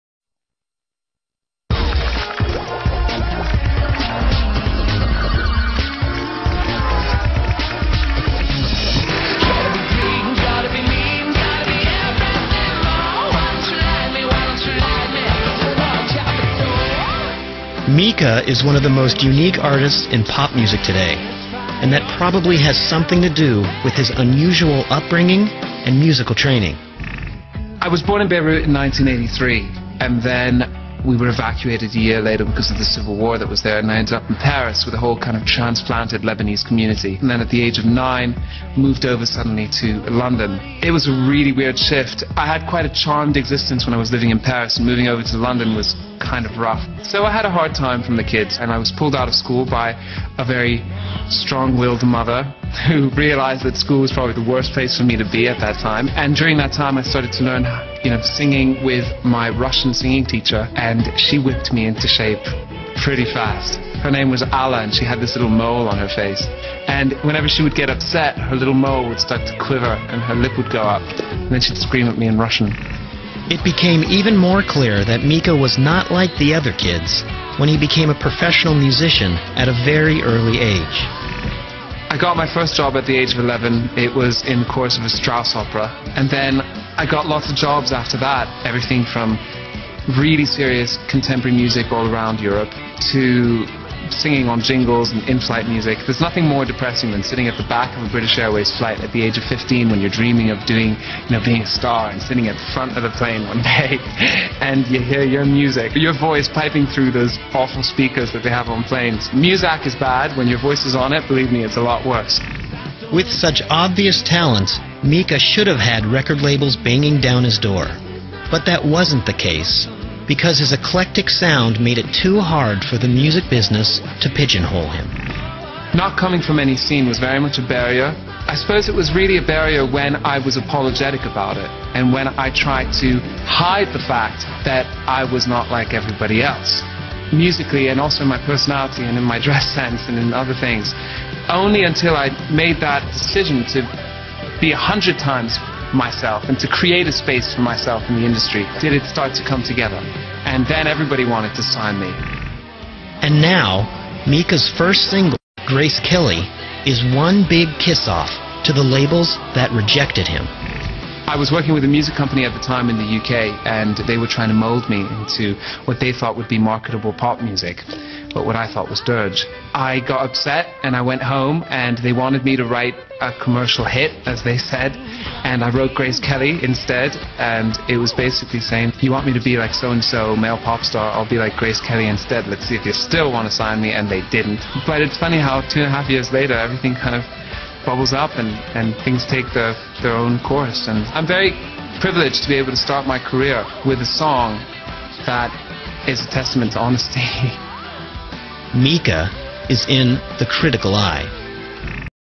Mika - US Interview (MikaObsessed)
Mika discussing his background and career.

March, 2007.